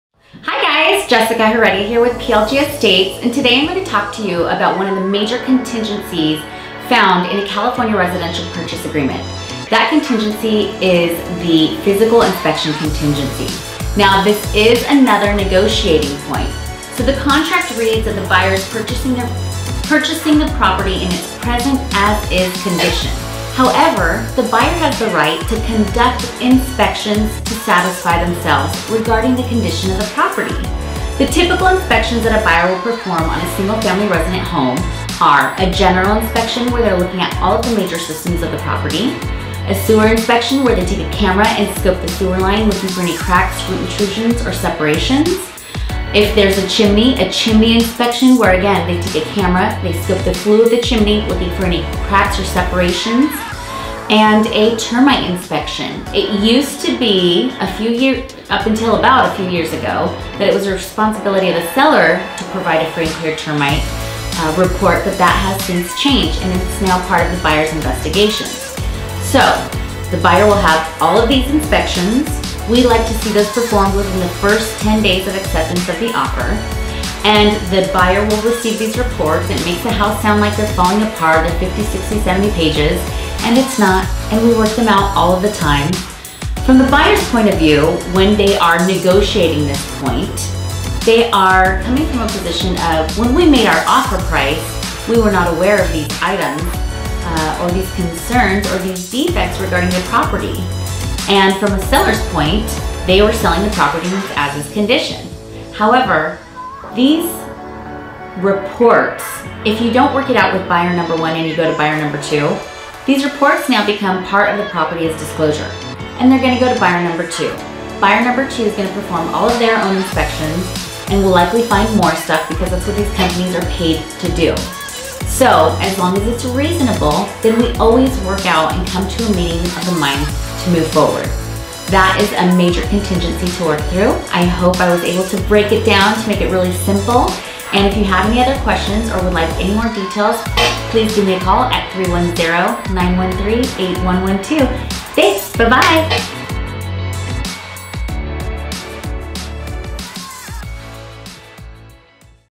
Inspection Contingency!
In this week's topic, Jessica Heredia reviews the Inspection Contingency in the California Residential Purchase Agreement and what that process looks like!